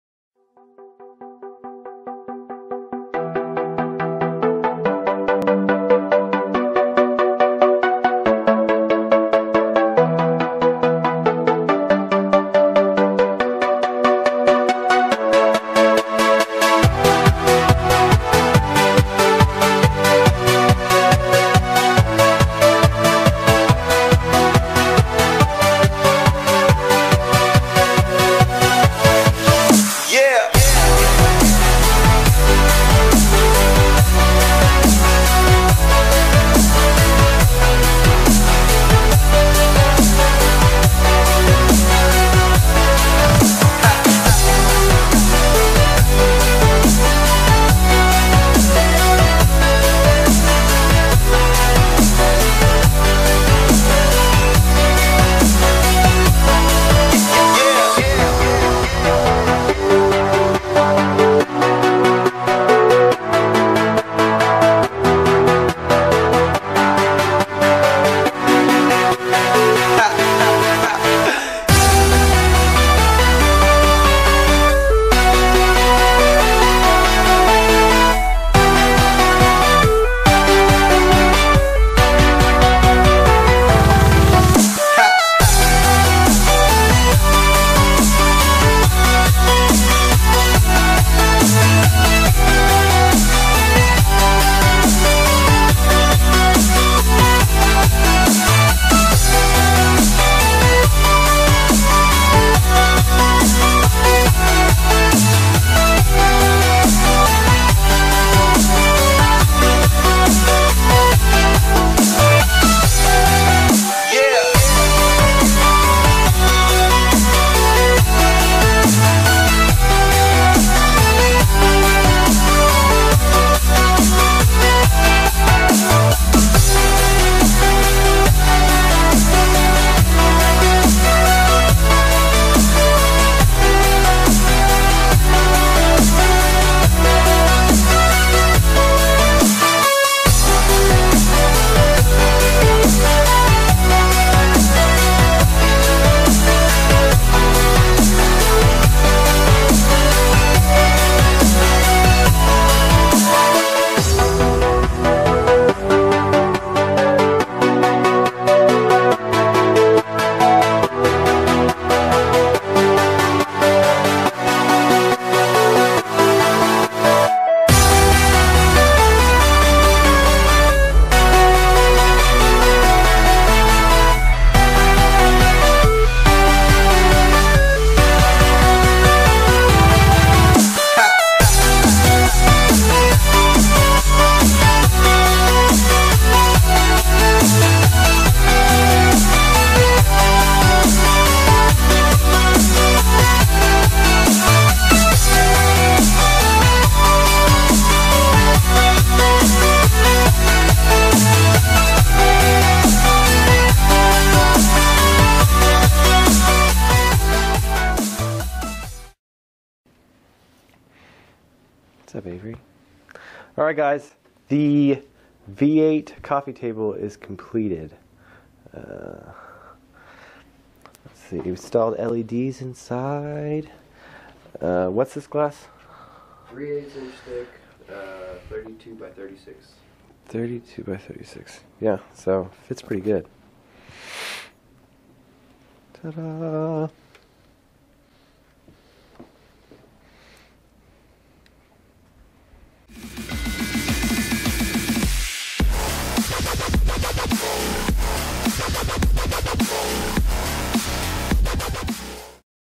V8 coffee table | DIY
My friend had an old engine that had some cracked cylinders so we turned it into a coffee table for his living room. Enjoy!